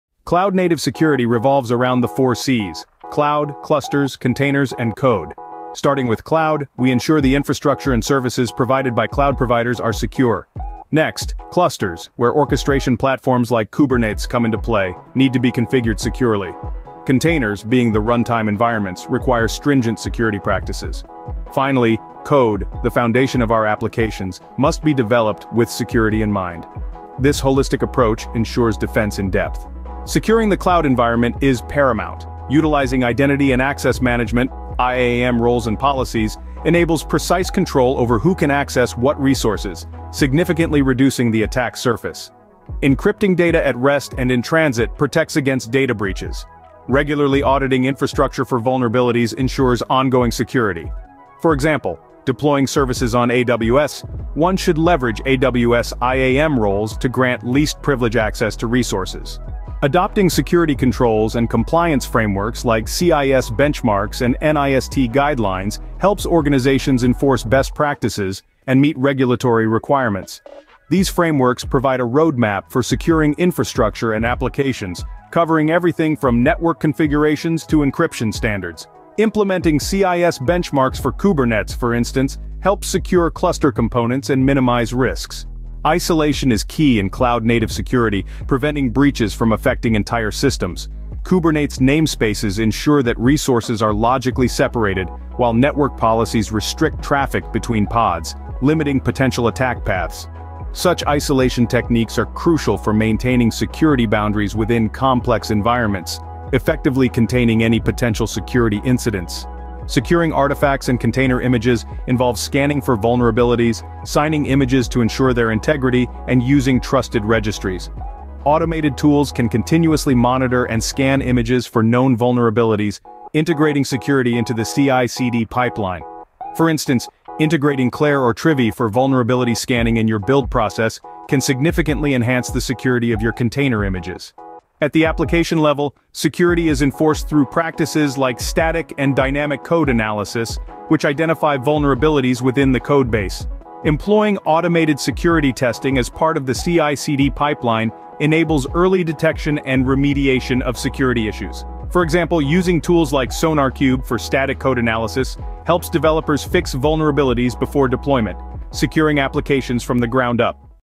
Kubernetes & Cloud Native Security Associate Exam Course - Lesson Overview of Cloud Native Security
In this video, we delve into the intricate world of software development, covering everything from project management to open-source licensing. Whether you're a beginner or looking to deepen your understanding, this video breaks down complex concepts into easy-to-follow explanations. Join us as we simplify the key aspects of software development that every enthusiast should know.

OUTLINE:

00:00:00 Software Project Management
00:00:31 Software Application Architecture
00:01:04 Functional Analysis
00:01:36 Open-source Software and Licensing